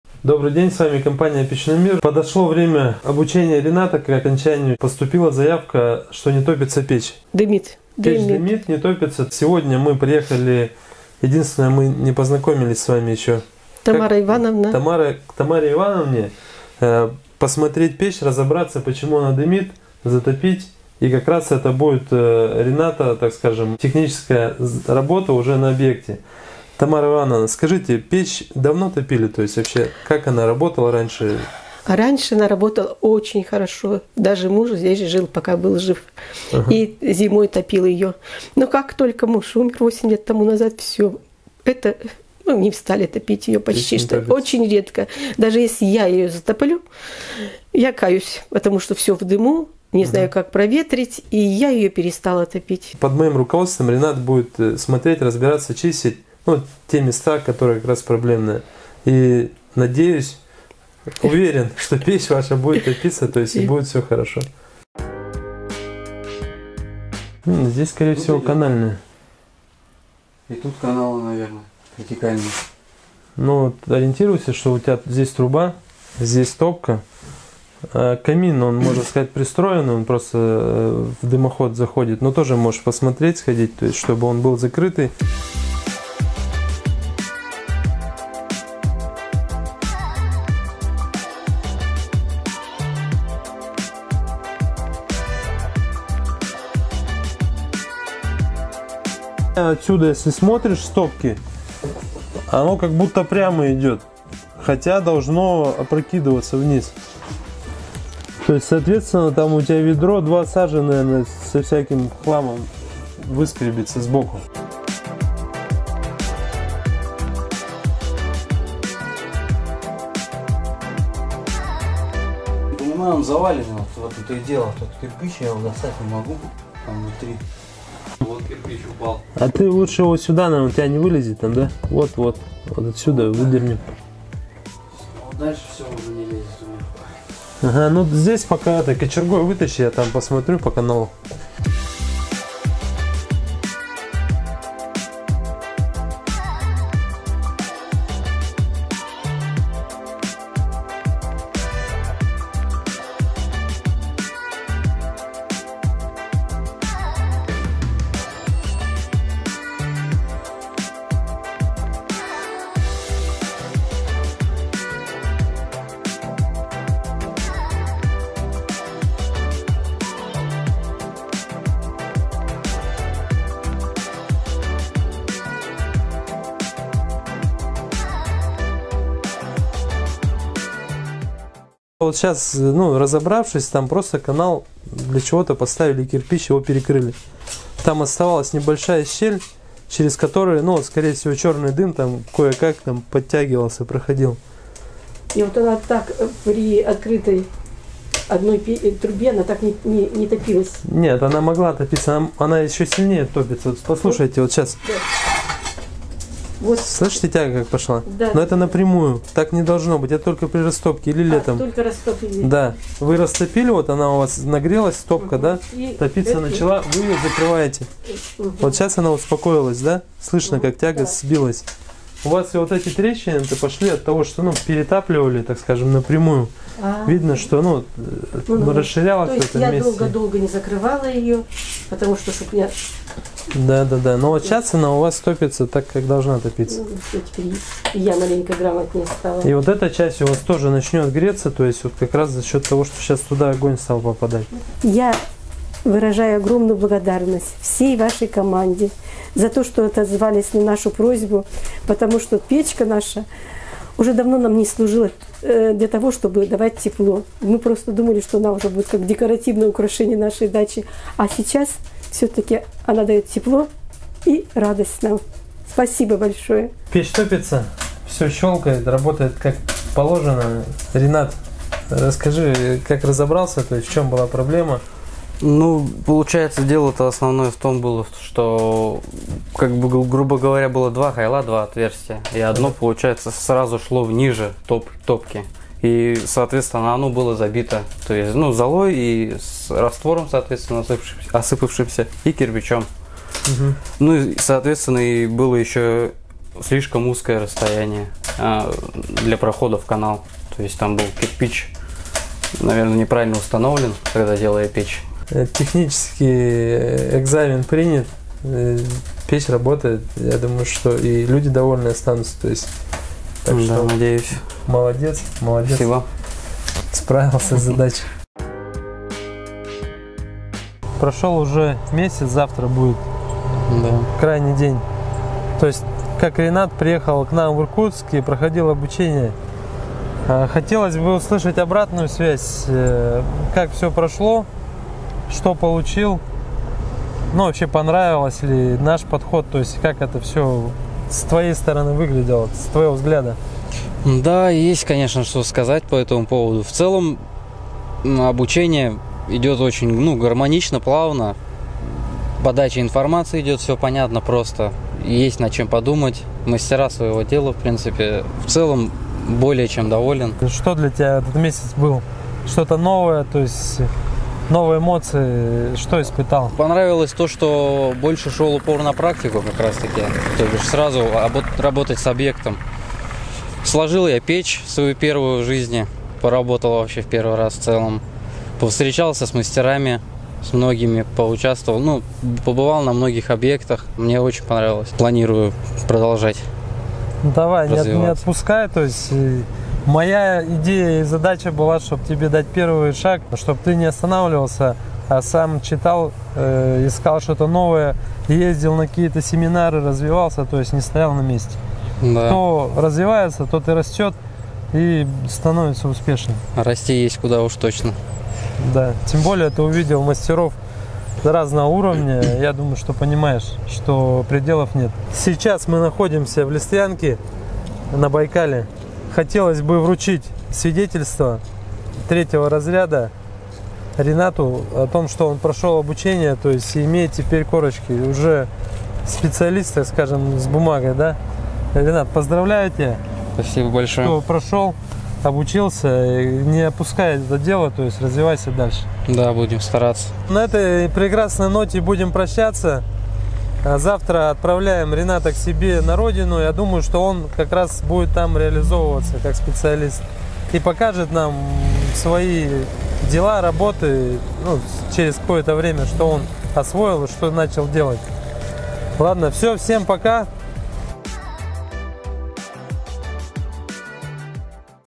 Ремонт печи за два часа: прочищаем канал и заменяем кирпич // Курсы печников
Публикуем третье и заключительное видео с Ринатом Кагармановым. В этом выпуске Ринат выполняет бесплатный ремонт печи в качестве экзамена на курсах печников.

Предыстория: в марте мы объявляли конкурс для тех, кто хочет бесплатно обучиться печному делу. Победил 24-летний житель Забайкалья Ринат Кагарманов. Ринат является инвалидом по диагнозу эпилепсия, а поэтому путь во многие профессии и компании для него закрыт. Печное дело - большой шанс для парня. Возможность обрести профессию и обеспечить себя и свою семью. В апреле Ринат приехал в Иркутск и приступил к обучению.

Наши контакты